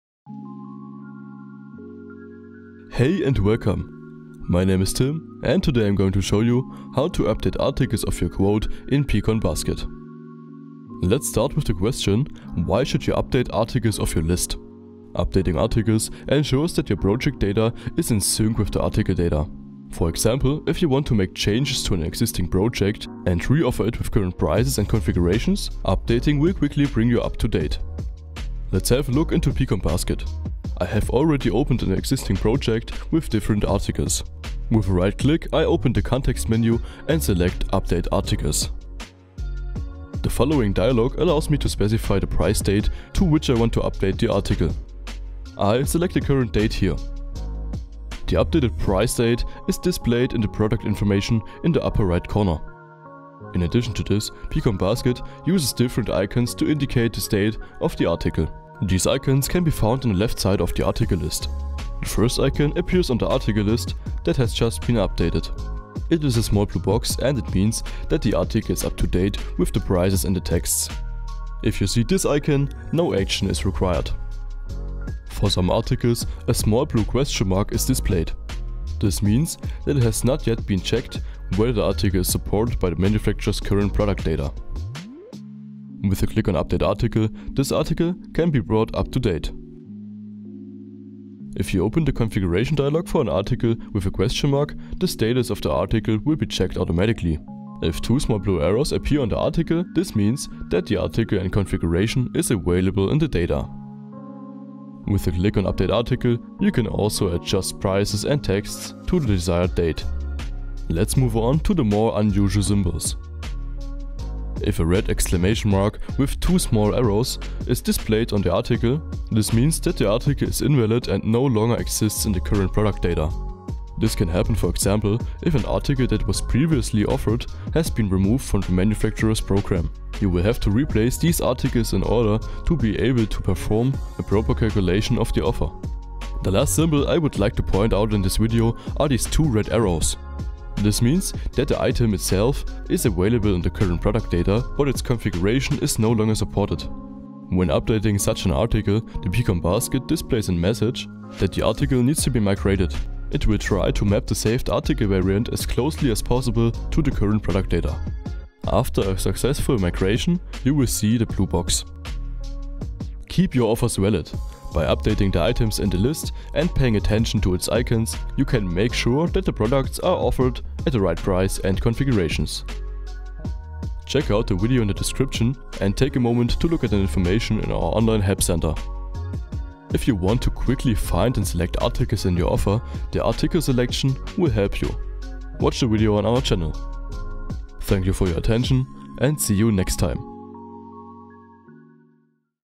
Why you should UPDATE ARTICLES on your offer | pCon.basket Tutorial
Make your offers binding! By updating the items in the list and paying attention to the icons, you can be sure that the right products are being offered at the right price.

0:00 Intro
0:27 Update Articles
0:53 Icons
2:42 Outro